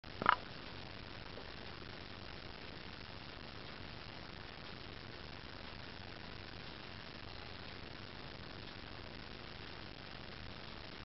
Hummingbird Feeder 07
A hummingbird stops by the feeder to refuel.